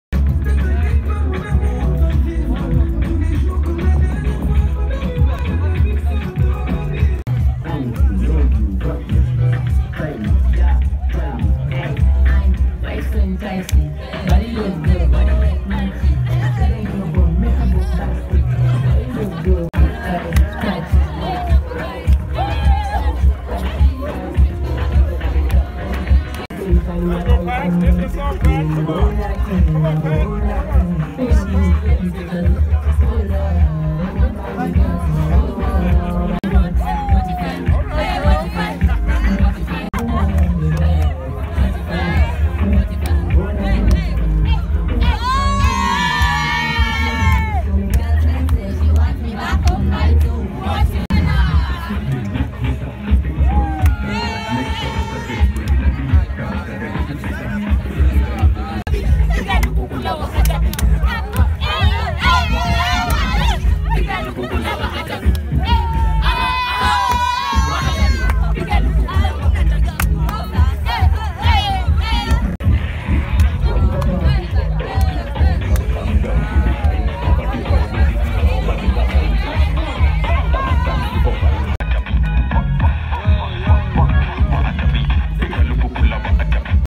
Dagoretti High Basketball Court Unveil by Giants of Africa - Kenyan Basketball
Welcome to Woopy Sport